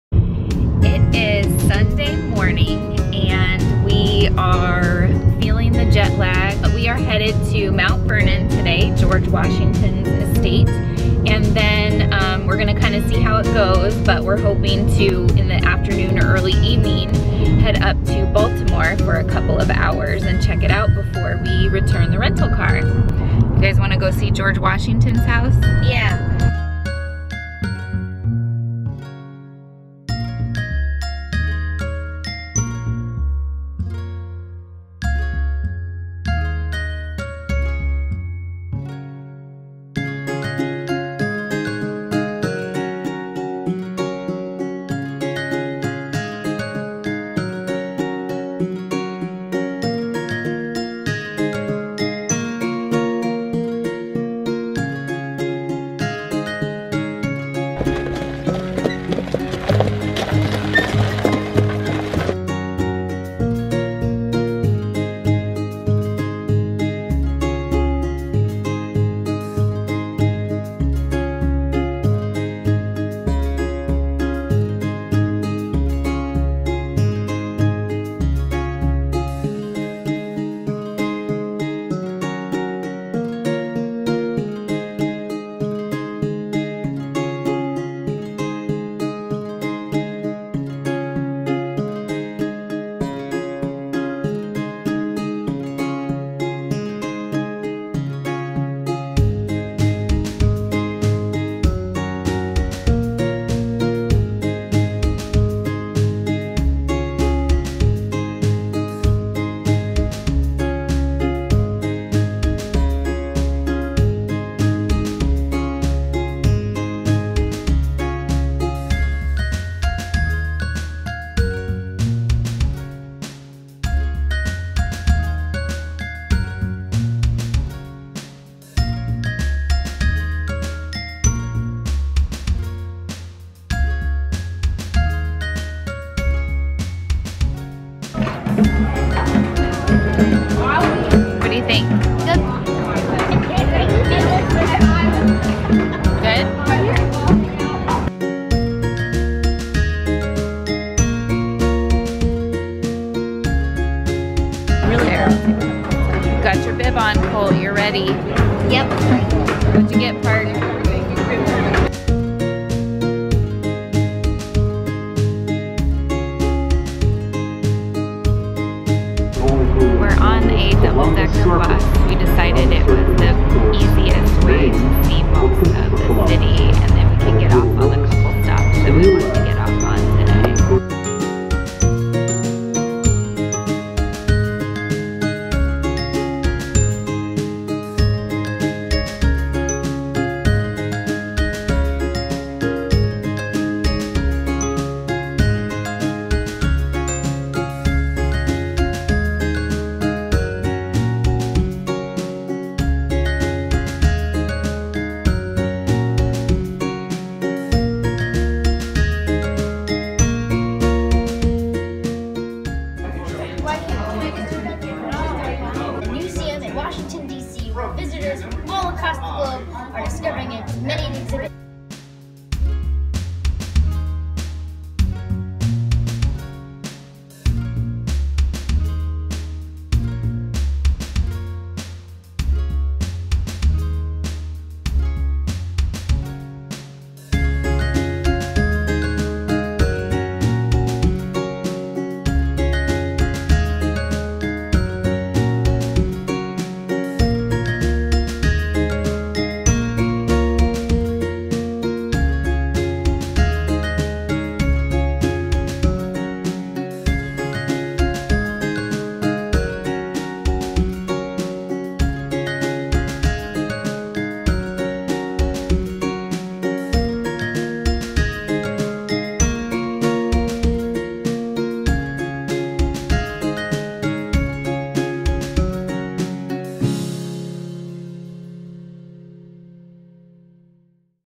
Family Travel to Washington DC Days 4-6
In this video we head to Mount Vernon, Baltimore (to explore some ships and seafood) and finish off our visit to Washington DC. We had such a great trip and I'm so glad we were finally able to make it happen!

I am so excited to share our recent trip that has long been in the works. This was one of those trips we've all be so excited to do and it feels so surreal that we were just there earlier this week!

This is days 1-3 where we explored Alexandria, VA, the Potomac, Washington DC including the National Museum of Natural History, National Archives and the Washington Monument, the Spy Museum, Arlington Cemetery and Georgetown.

We were given a media discount and/or tickets but were not compensated or required to post or create content. All opinions are our own!